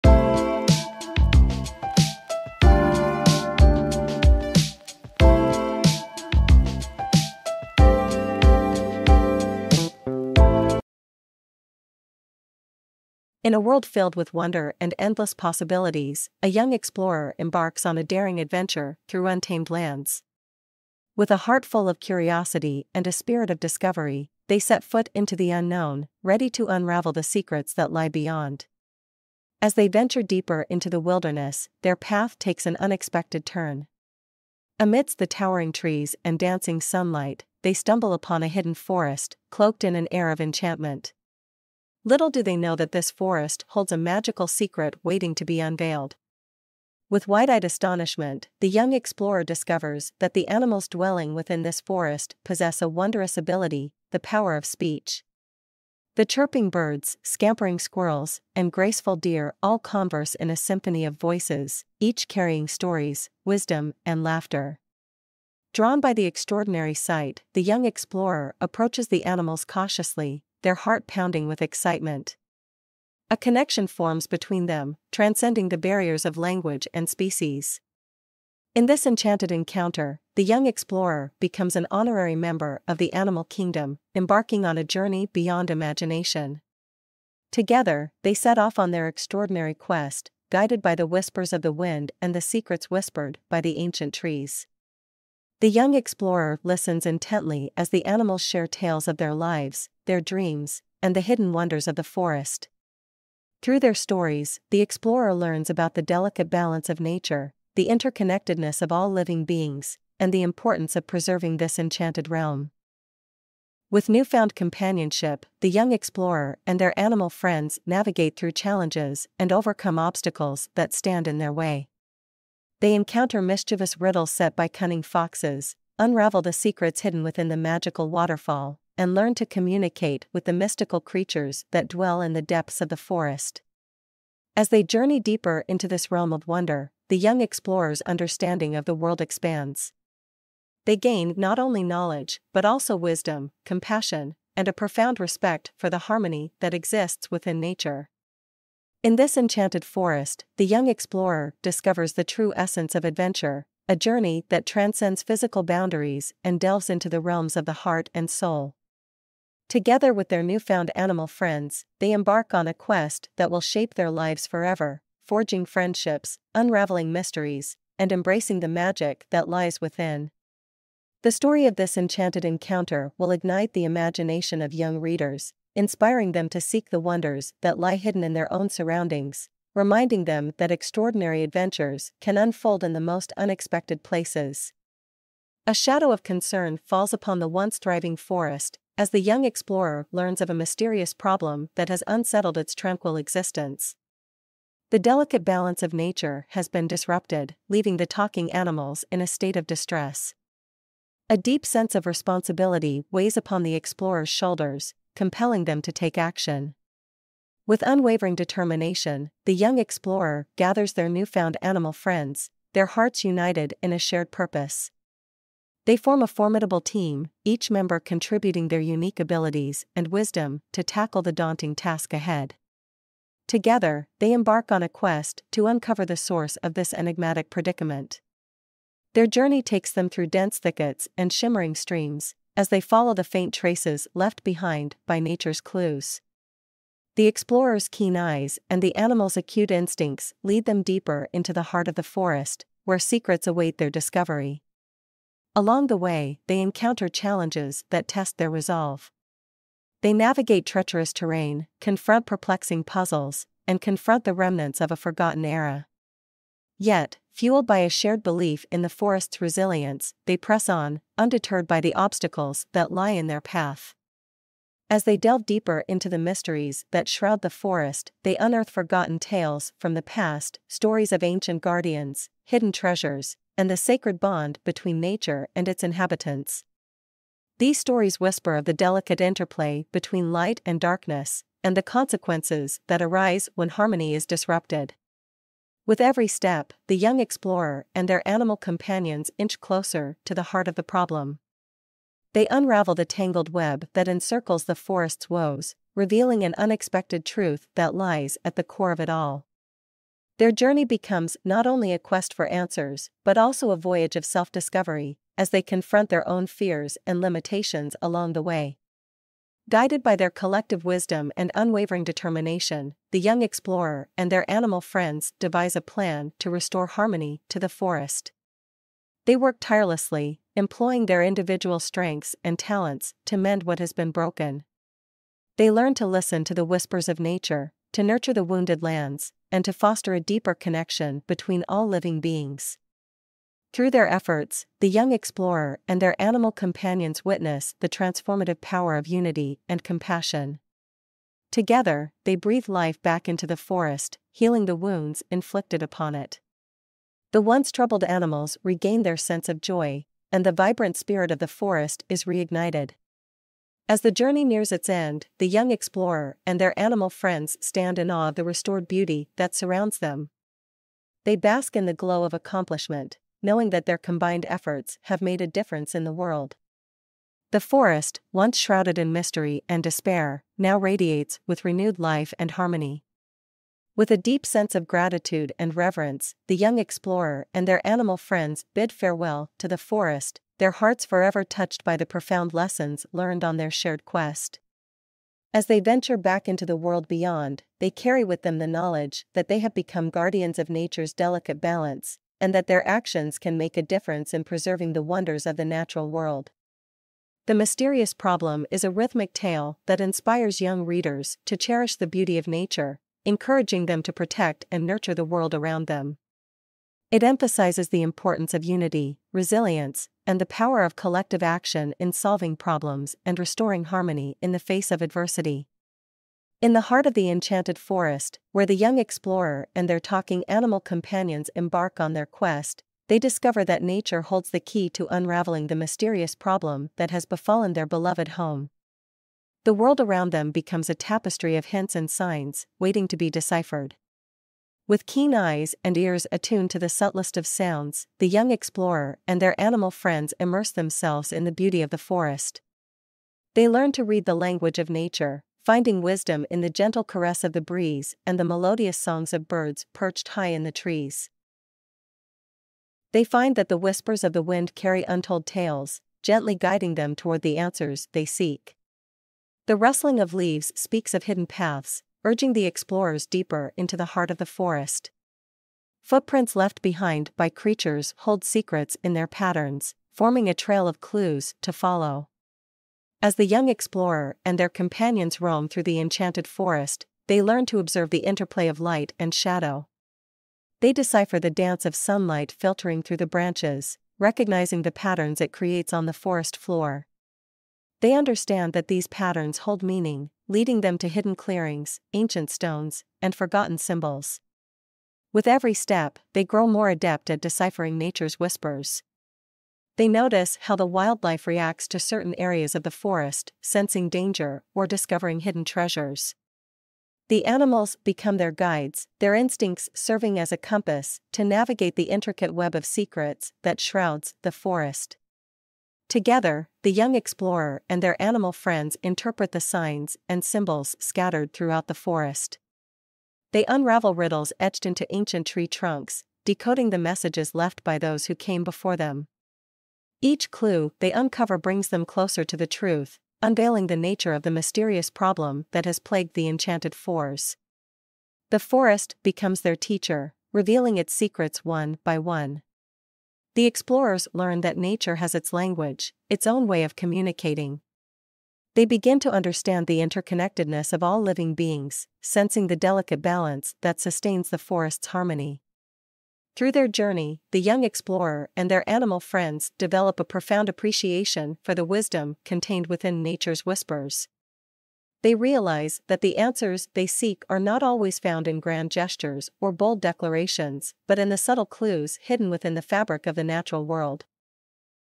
"The Whispering Woods: A Journey of Friendship and Discovery"
The story highlights the importance of empathy, communication, and respect for others, as the young explorer and the talking animals learn to listen to one another and appreciate the diverse perspectives they bring. Through their shared experiences, they forge unbreakable bonds, demonstrating the transformative power of genuine friendship and cooperation.